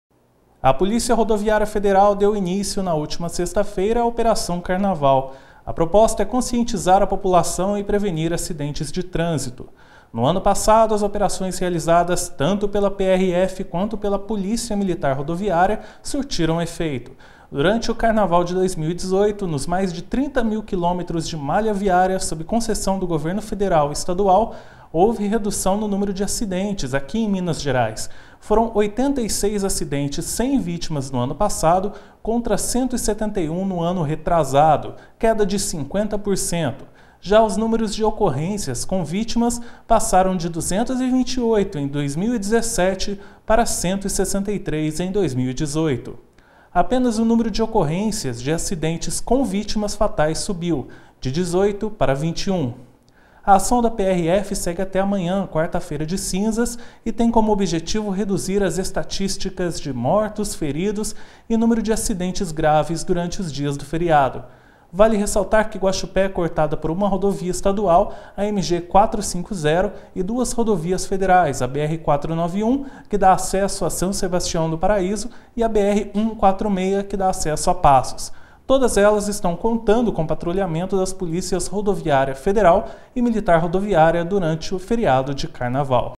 Polícia Rodoviária Federal e Polícia Militar Rodoviária dão início a Operação Carnaval
A proposta é conscientizar a população e prevenir acidentes de trânsito.